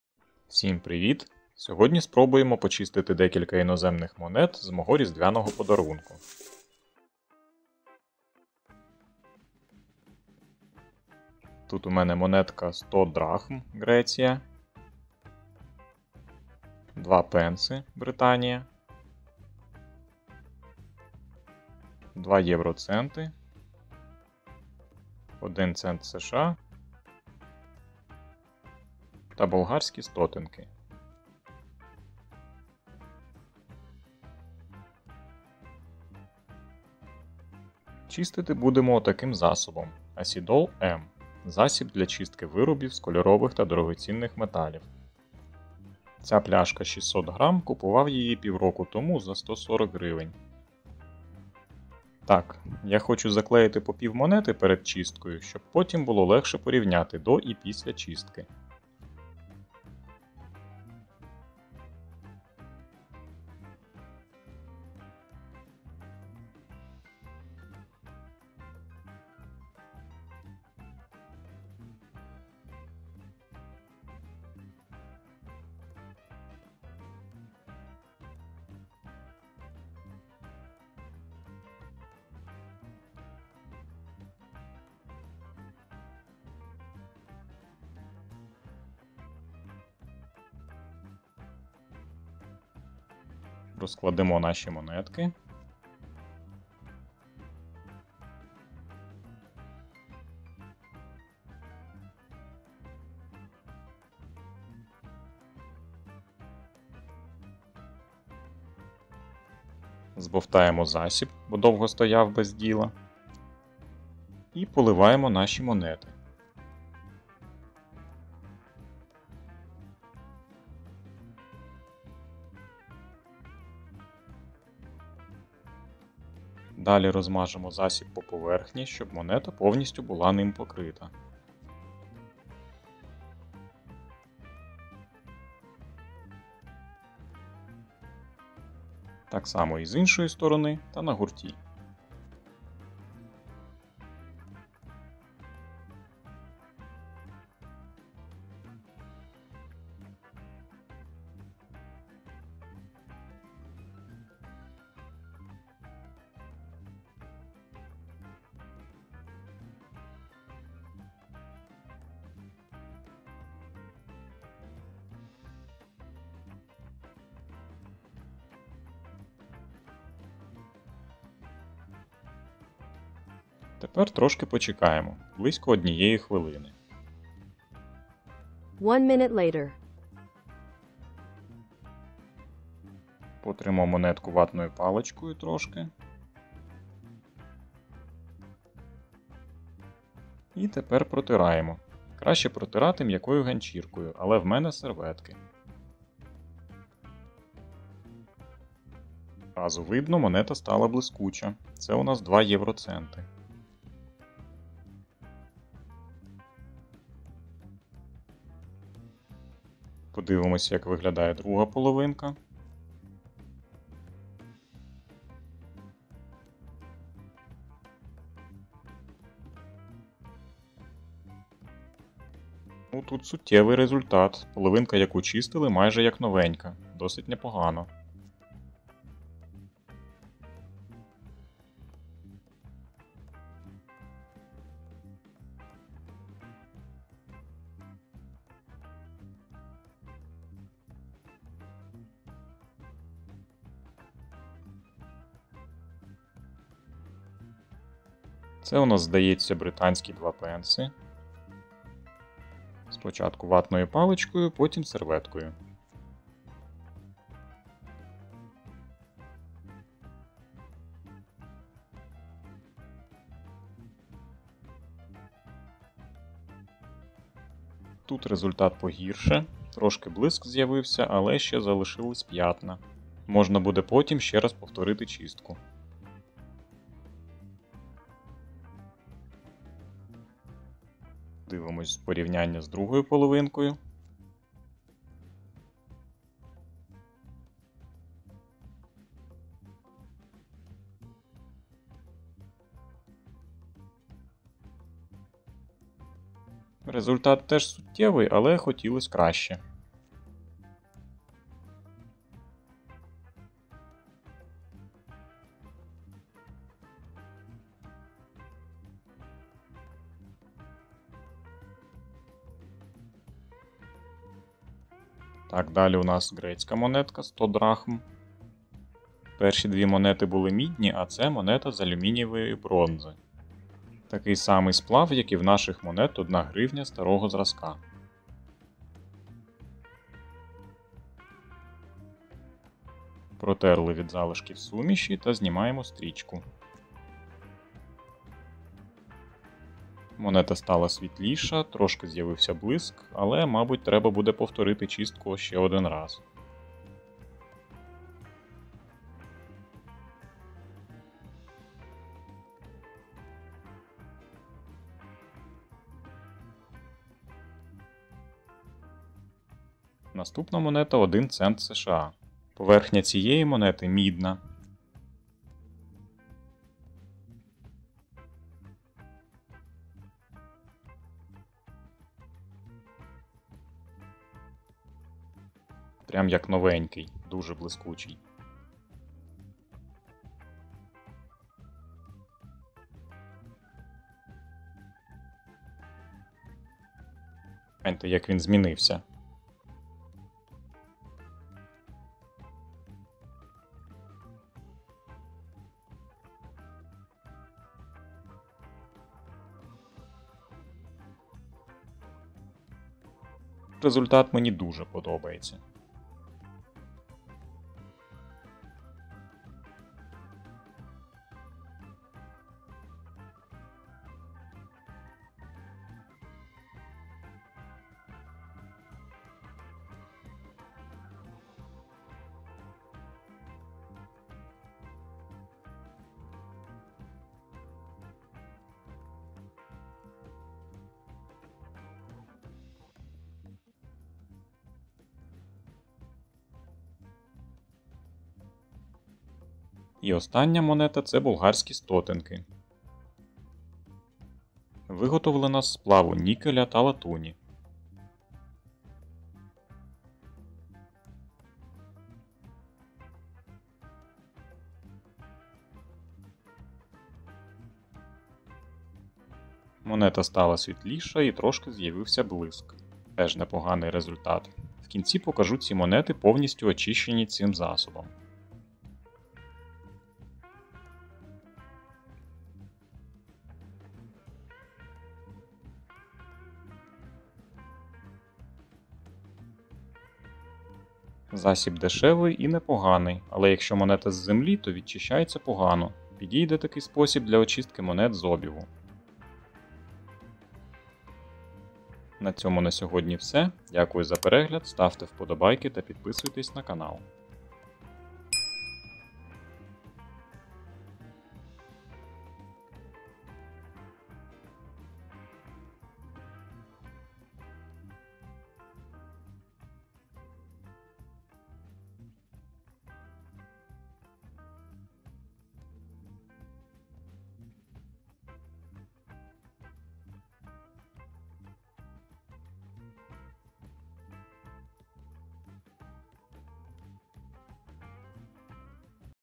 Чистка обігових монет
Чистка обігових монет, мідних, бронзових, нікель, латунь.